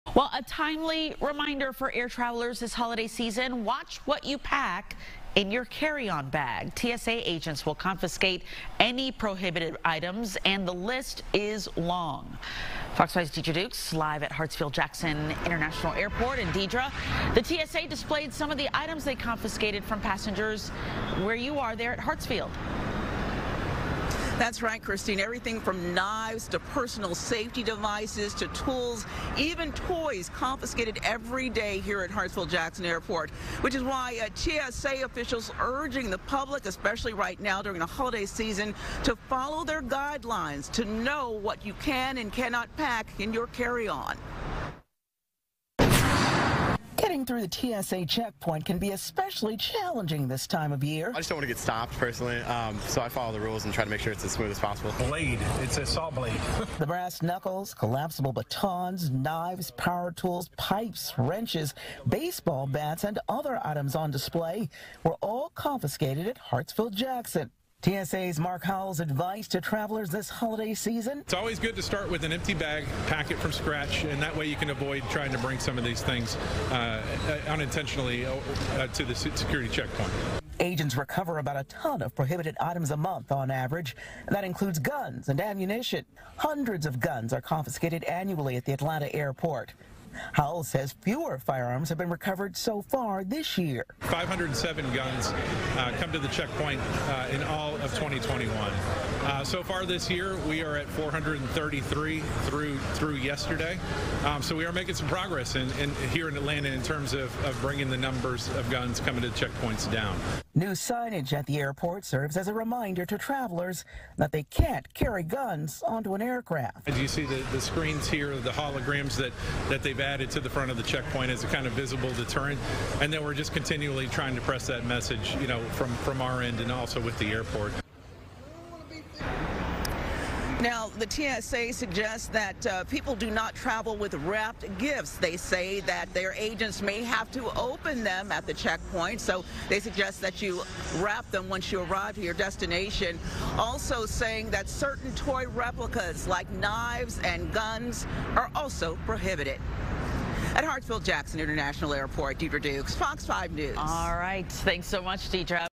TSA explains what not to bring on your next flight
Getting through the TSA checkpoint can be especially difficult during this time of year. Here's a list of items not to bring through security, some may surprise you.